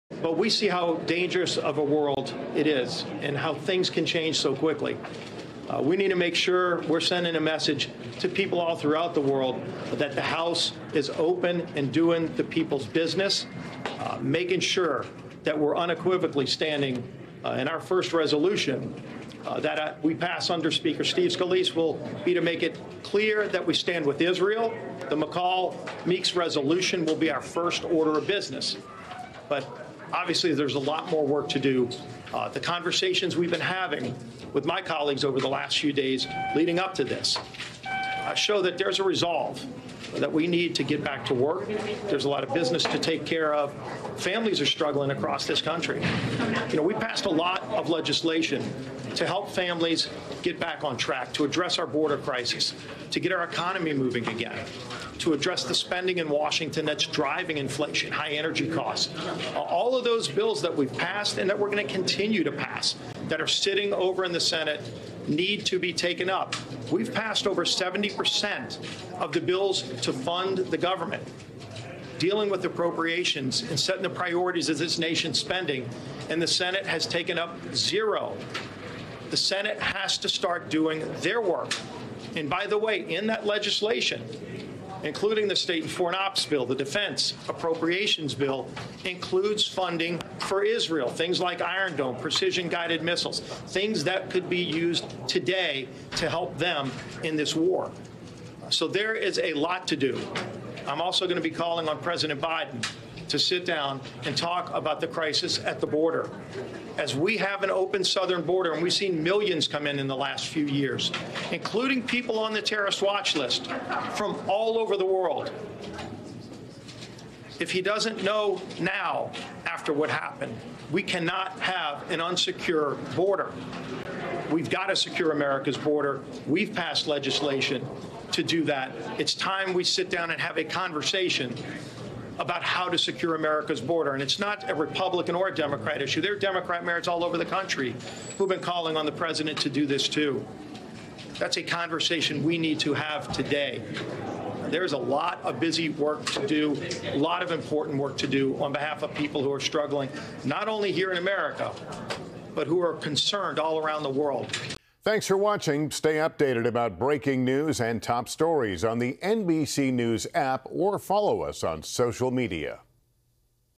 Rep. Scalise speaks after winning GOP nomination for House speaker
Rep. Steve Scalise, R-La., spoke to reporters after winning the Republican nomination to serve as the next House speaker and discussed what he hopes to prioritize.